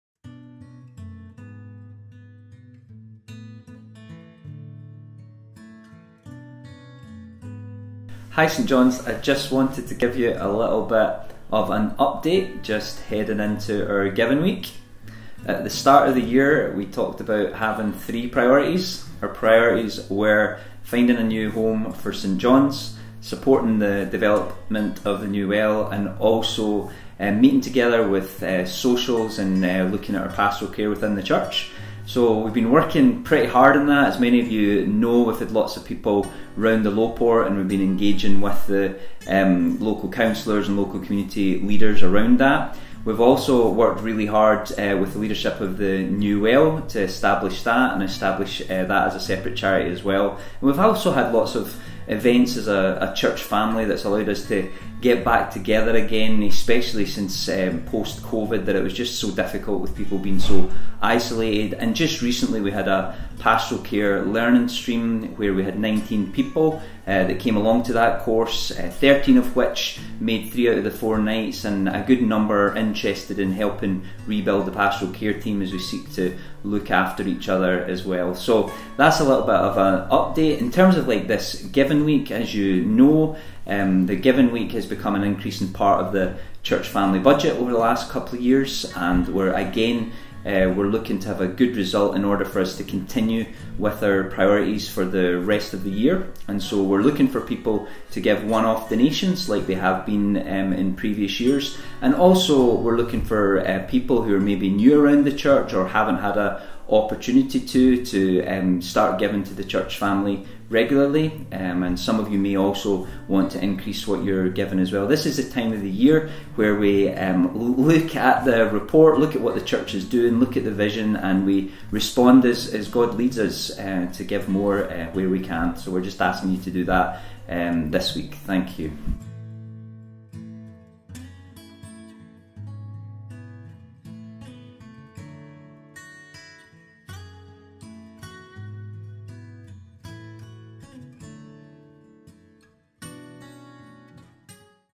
GIVING WEEK 2022: A word from Stuart
We are thankful for the continued generosity of all those who have given to the church this year. The Giving Week is an opportunity for you to give towards the vision of St John’s and also to meet the budget requirements that were agreed at the AGM in March

Please prayerfully consider giving a one-off gift, increasing your monthly giving or start giving